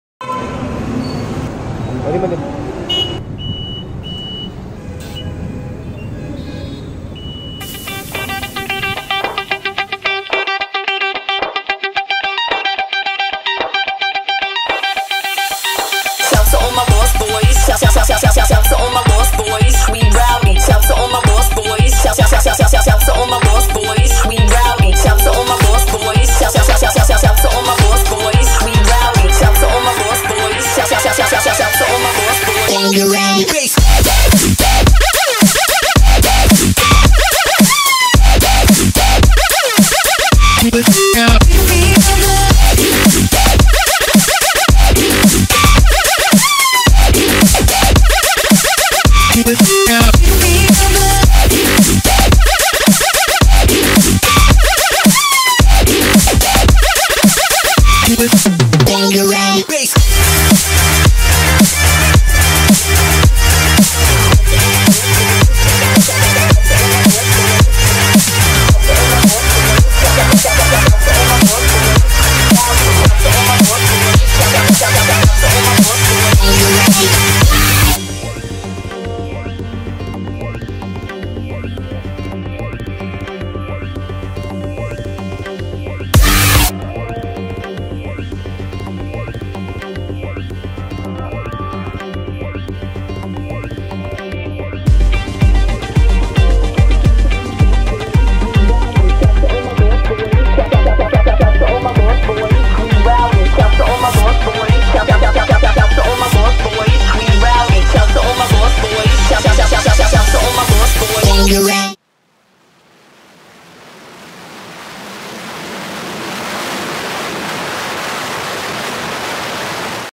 Nandi Hills Banglore - Ktm RC 390, Interceptor 650, Himalayan
Chennai to Nandi Hills Banglore bike trip on Ktm Rc 390, Royal Enfield Interceptor 640, Royal Enfield Himalayan